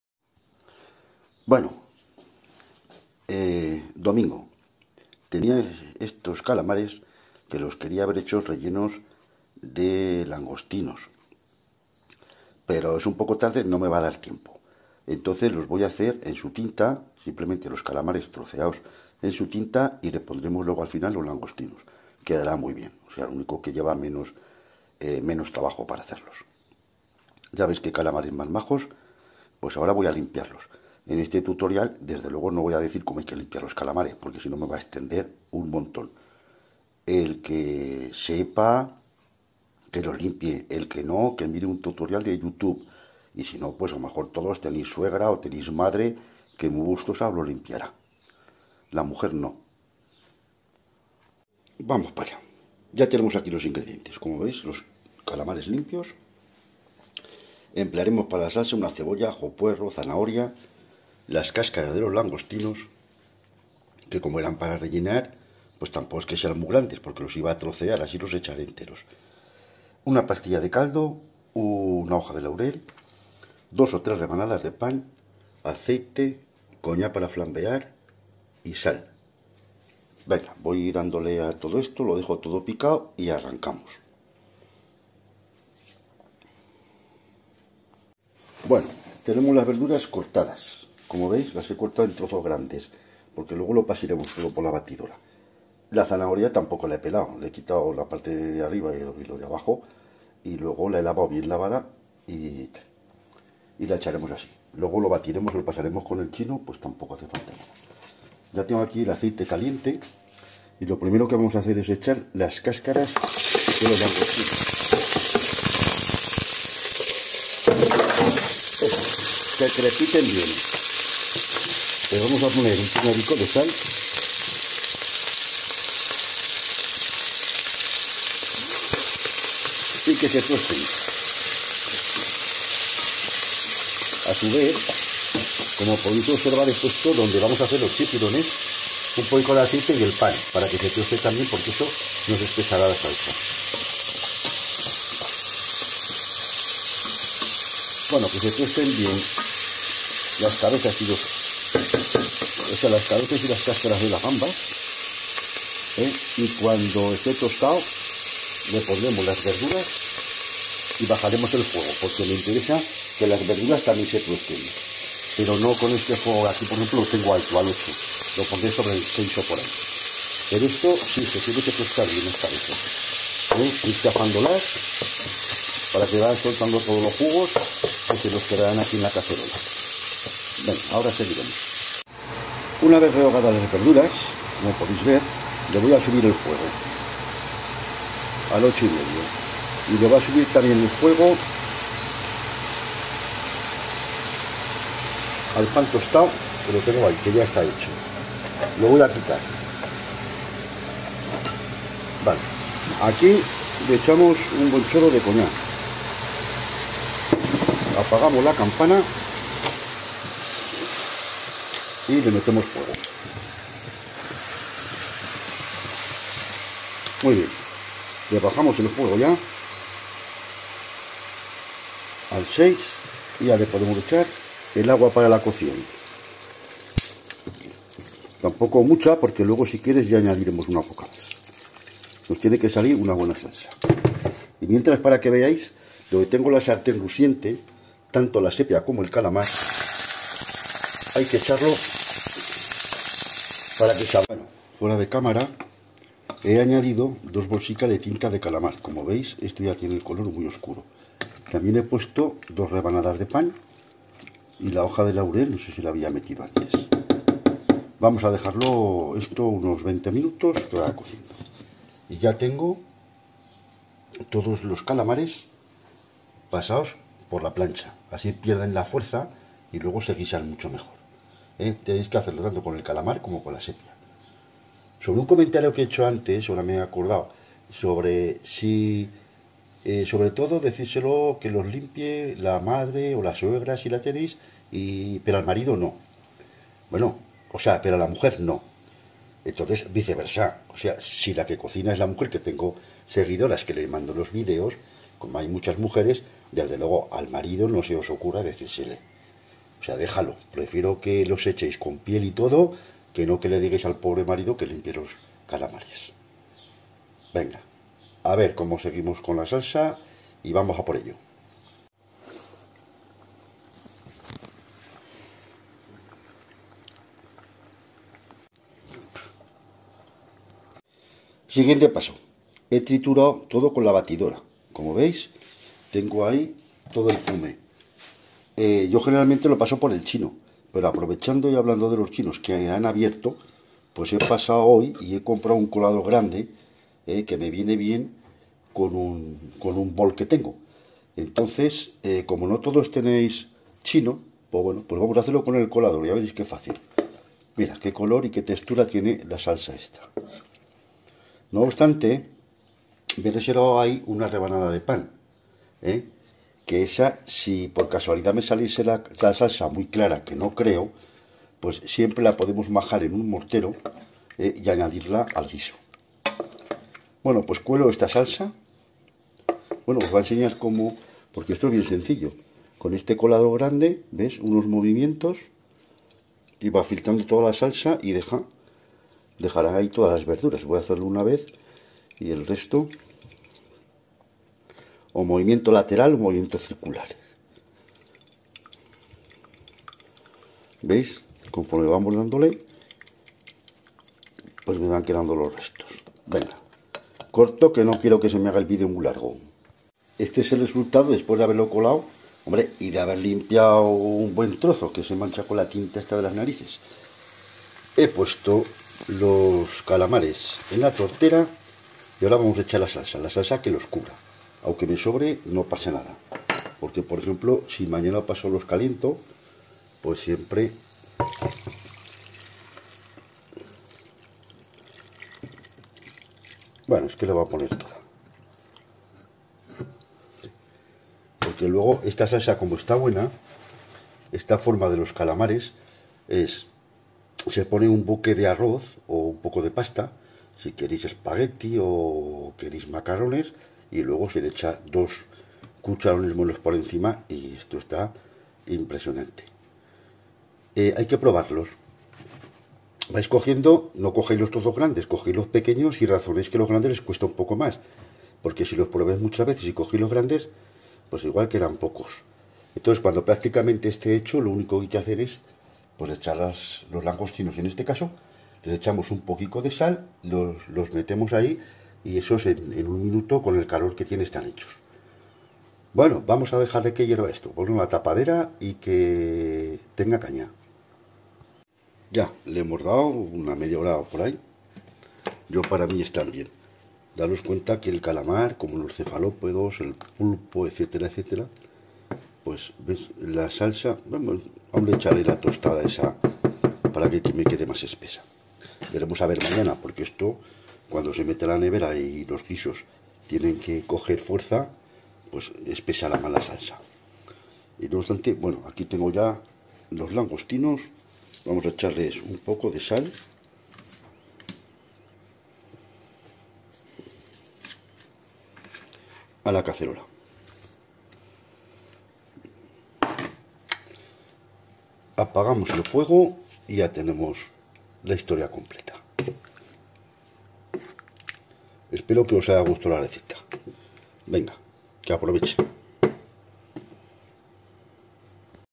Ésta receta sorprendió a mi familia. Calamares en tinta 👨🏻‍🍳 La cocina de Pipo.
👨🏻‍🍳 Os enseño a hacer calamares en tinta.
Si los has hecho y te han gustado déjame un comentario.